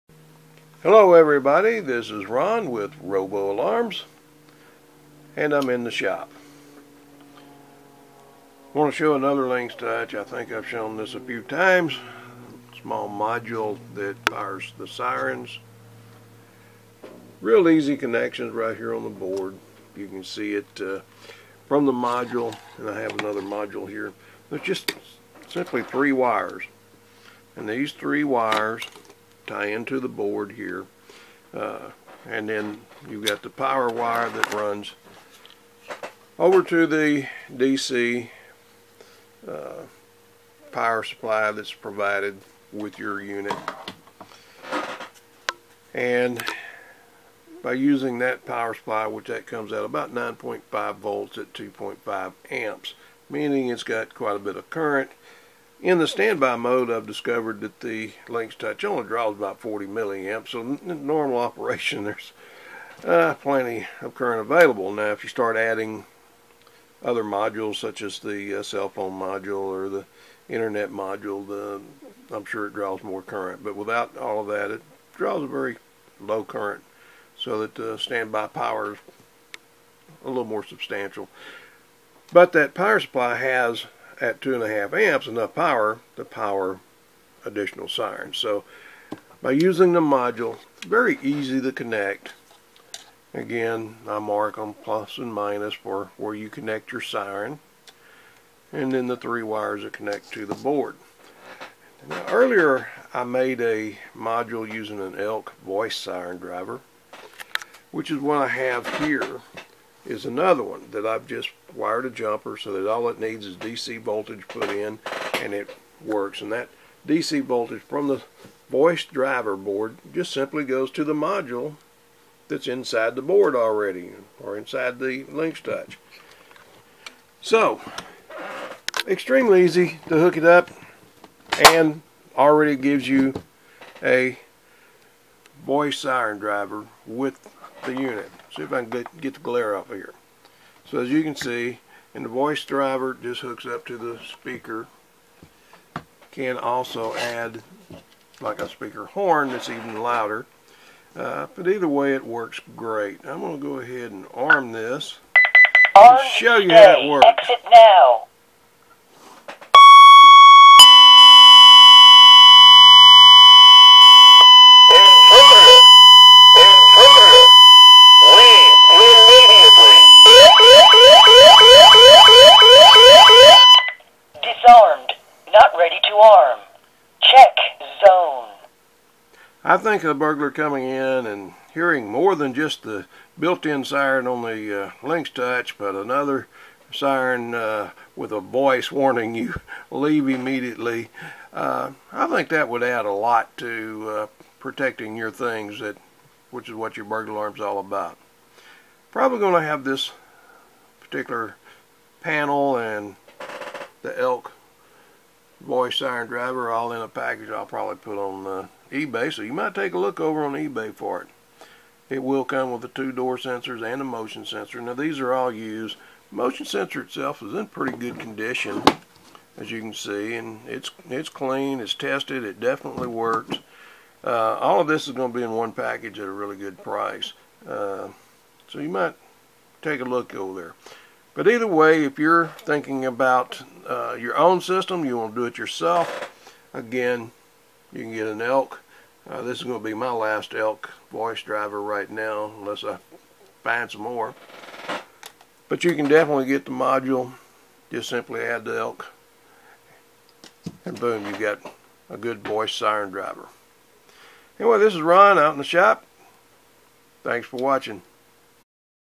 Ron in the shop - Elk Voice siren driver for DIY Honeywell Lynx Touch Alarm system
For a DIY alarm system that can use the ELK voice siren with a Honeywell Lynx Touch. No extra power supplies simply add the siren module and connect it to the Elk Voice siren driver and power it all from the Lynx 9vdc power supply that your alarm system came with.